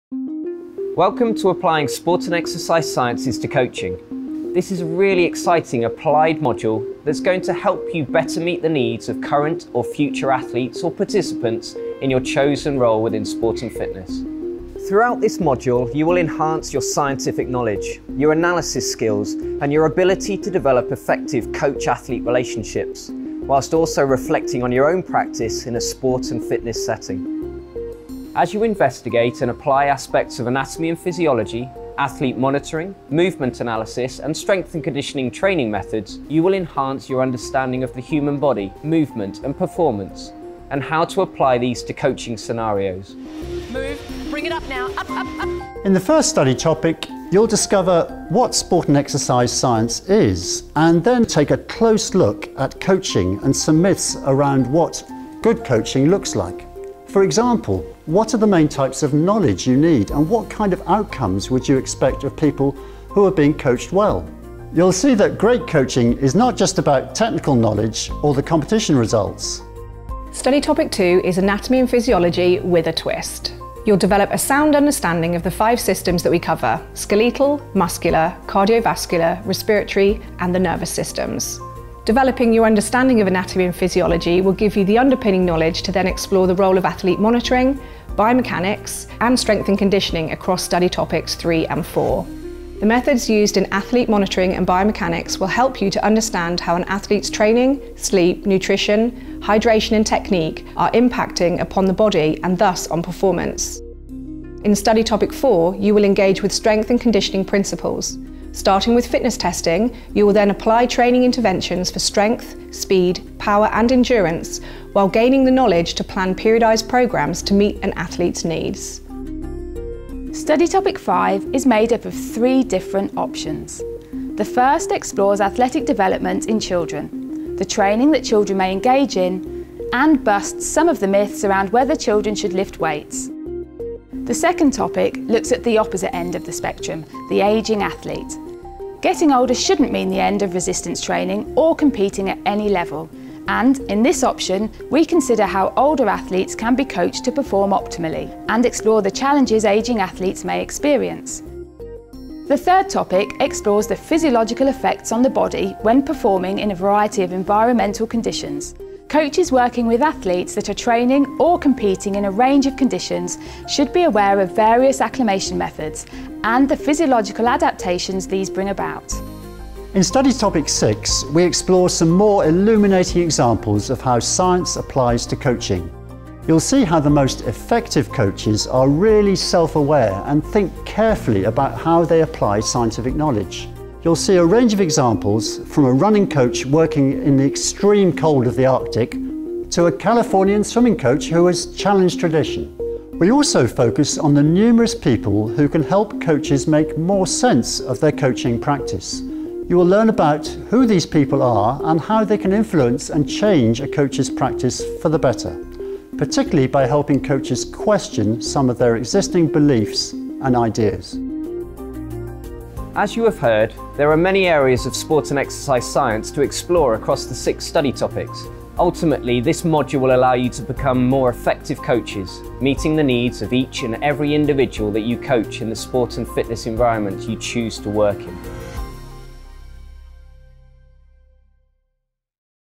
The Open University: E236 Applying Sport and Exercise Science to Coaching
The Open University: An overview of the E236 Applying Sport and Exercise Science to Coaching module content on the BSc Sport and Fitness programme.